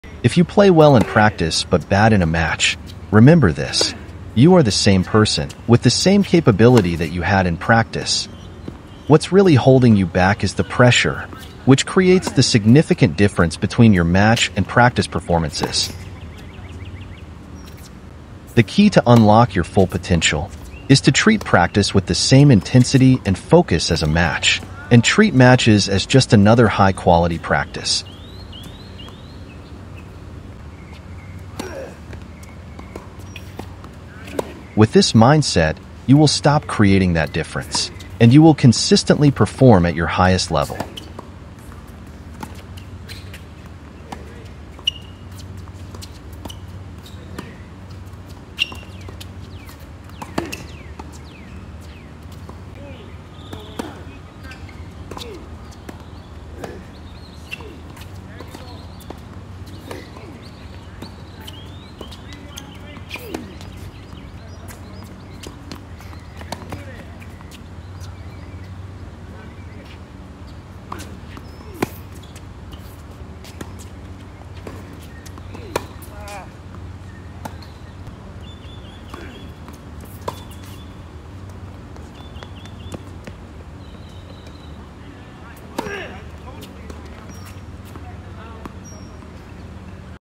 SL vs CC Tennis Highlights. 🎾 The Tennis Paradox: Practice vs. Matches 🎾
Ever find yourself playing like a pro in practice but struggling to perform in matches? You're not alone! The transition from practice to competition is one of the toughest challenges for any tennis player. 🧠💥

In practice, we’re relaxed, experimenting with shots, and building confidence. But once the match starts, nerves kick in, and the pressure can change everything. It's a mental game as much as a physical one. 🎯✨

Here are a few tips to bridge the gap:

-Simulate Match Conditions: Practice with the same intensity and focus as you would in a match.
-Mental Toughness: Work on your mindset. Visualization and breathing exercises can help manage stress.
-Consistency Over Flash: Focus on playing steady, high-percentage tennis rather than going for risky shots.

Remember, improvement takes time and persistence. Keep pushing, stay positive, and trust your training. Let’s turn those practice wins into match victories! 💪🏆